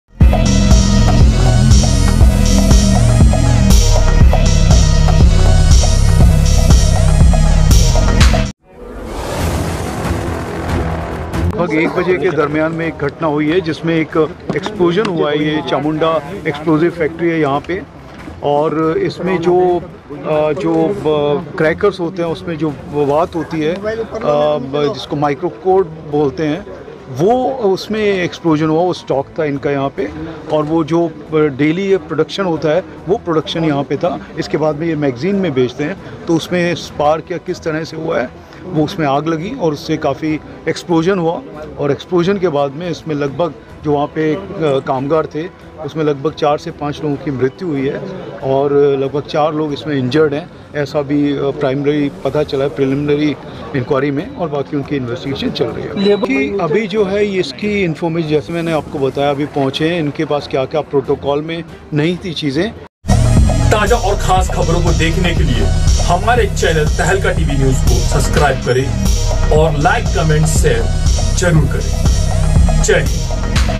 चामुंडा बारूद फैक्ट्री में हुआ ब्लास्ट 5 की मौत चार लोग घाय
चामुंडा बारूद फैक्ट्री में हुआ ब्लास्ट 5 की मौत चार लोग घायल